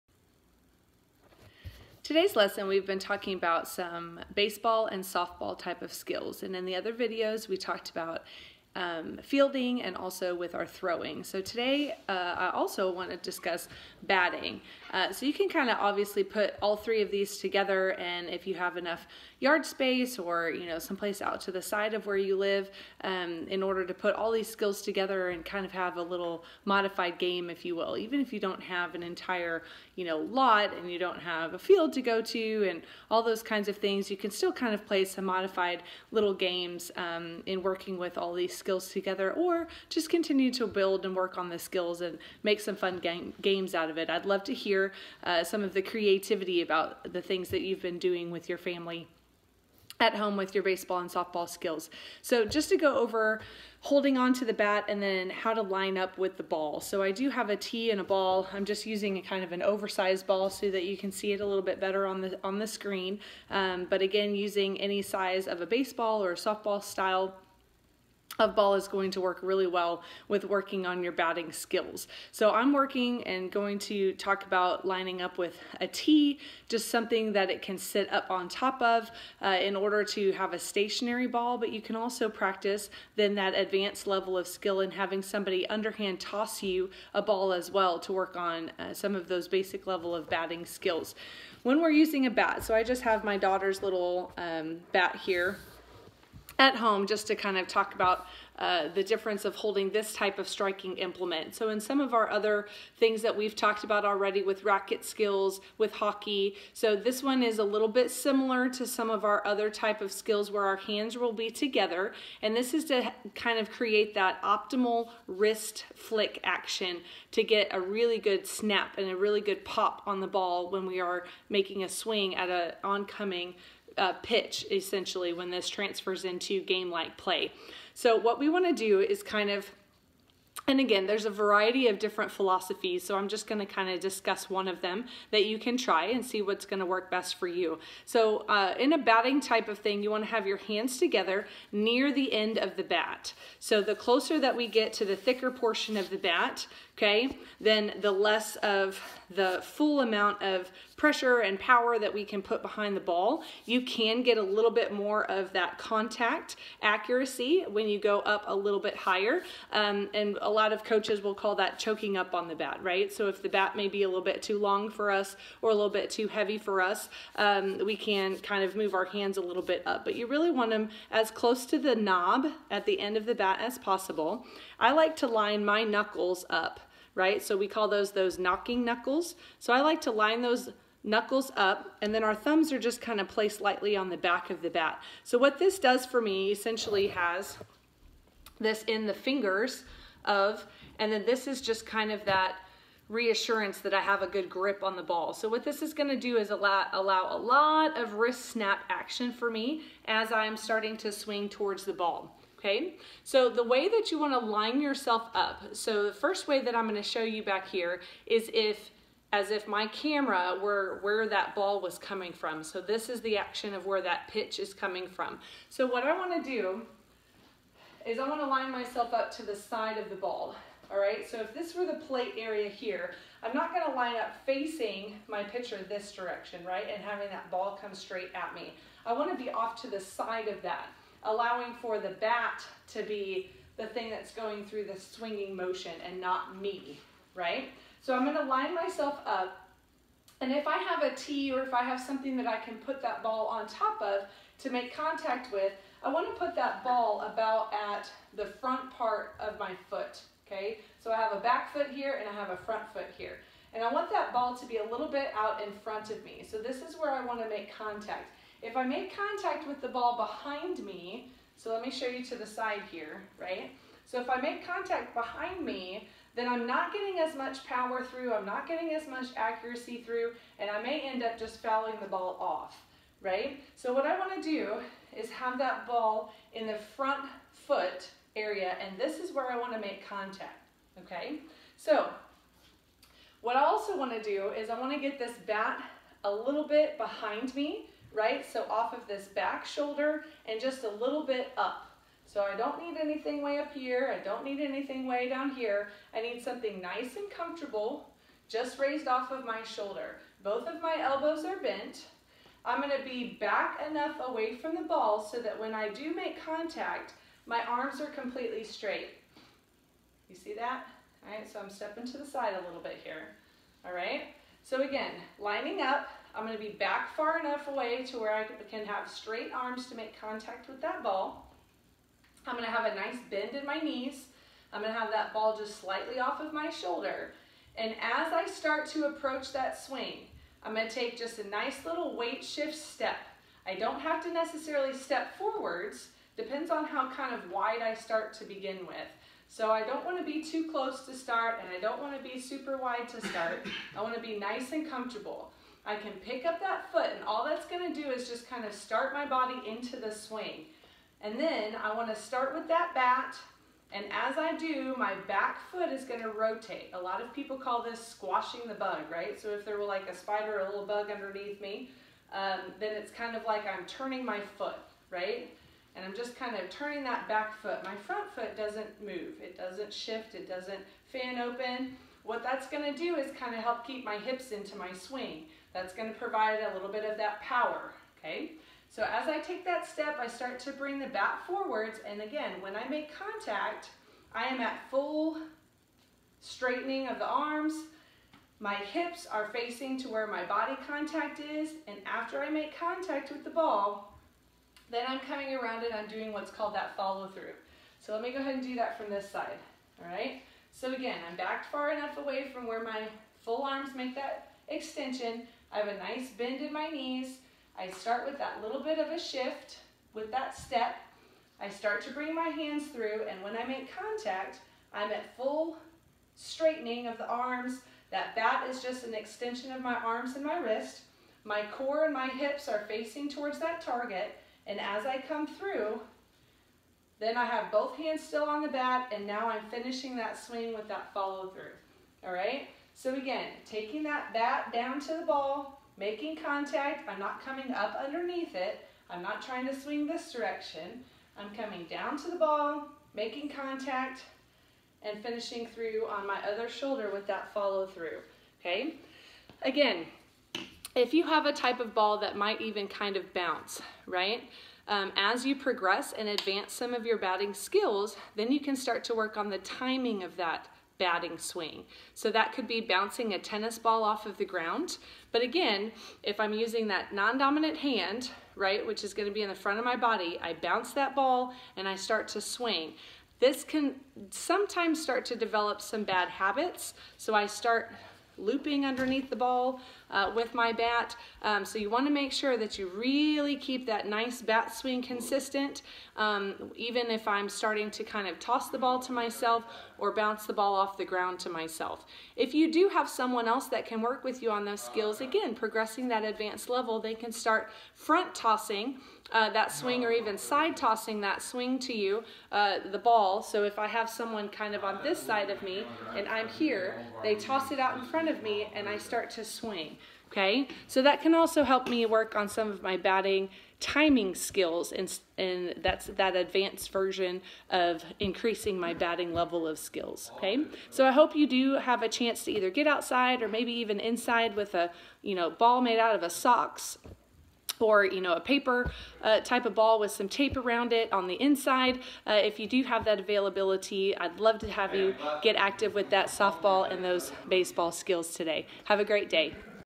Baseball/Softball Skills: Batting
This video gives a basic breakdown of a batted swing, starting with grip and placement over a plate or batting tee. Concepts will also include different drill practices and indications for advanced skills.